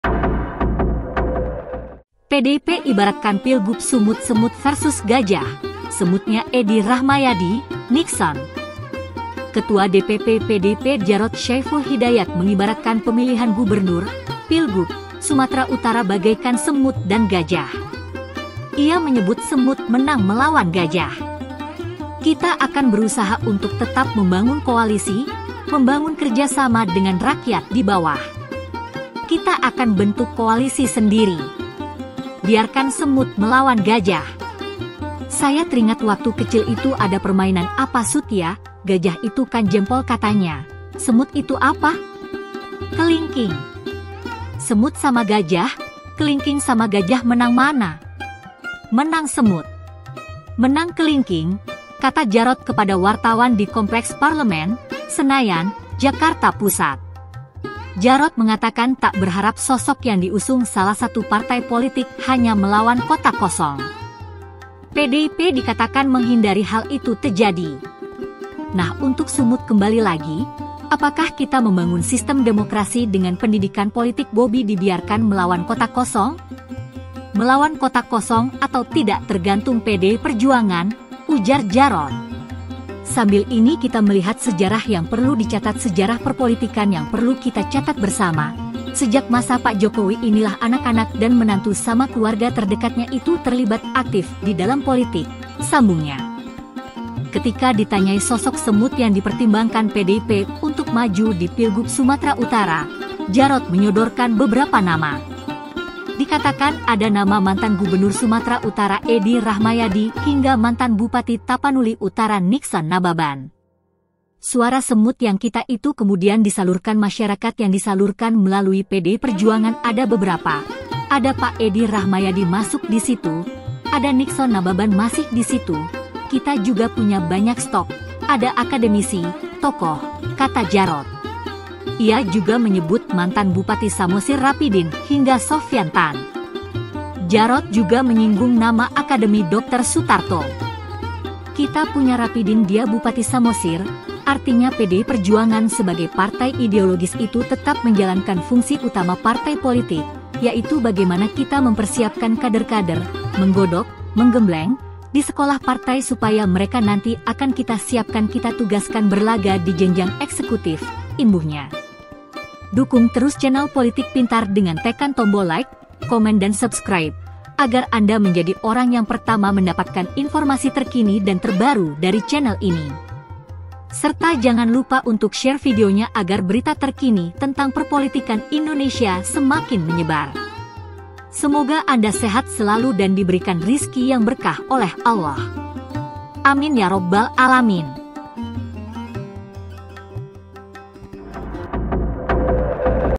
PDIP Ibaratkan Pilgub Sumut Semut Vs Gajah: Semutnya Edy Rahmayadi, Nikson
Ketua DPP PDIP Djarot Syaiful Hidayat mengibaratkan pemilihan gubernur (Pilgub) Sumatera Utara bagaikan semut dan gajah. Ia menyebut semut menang melawan gajah.

Jangan lewatkan berita terkini 24 jam non stop di channel PolitikPintar. Aktifkan juga lonceng supaya kamu dapat notifikasi video terbaru dari PolitikPintar.